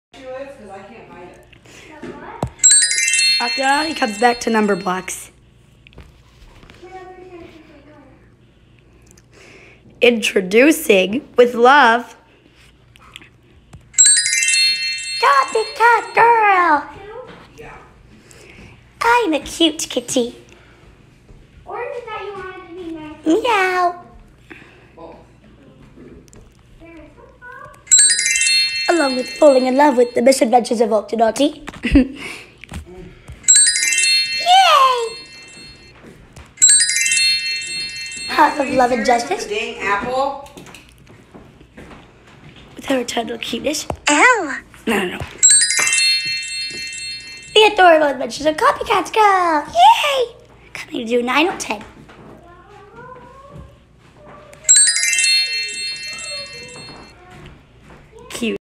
💖Octonaughty came back to Numberblocks and introducing CopyCat Girl (Read description)💖
Yesterday now in 3D came out and Octonaughty returned and came back to Numberblocks!!! Most exciting thing ever!!! Meet CopyCat Girl and you will see some cute pictures of Octonaughty. You will love the TOTEMO KAWAII picture of Octonaughty with big puppy dog or kitty cat eyes. You will REALLY LOVE the TOTEMO KAWAII picture of Octonaughty with crazy eyes. Enjoy the cuteness~!